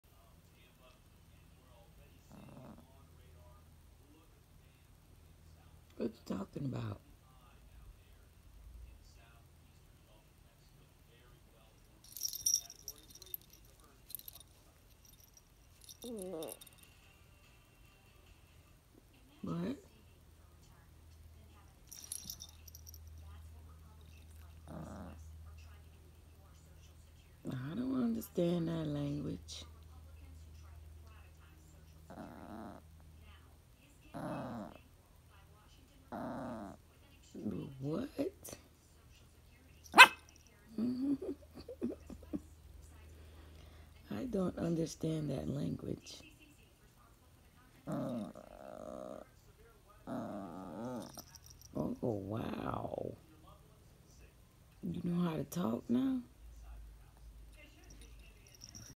WHO KNOWS WHAT THIS YORKIE IS TRYING TO TELL ME🤣🤣🤷🏾‍♀️
THIS CUTE LIL YORKIE IS TRYING TO TELL ME SOMETHING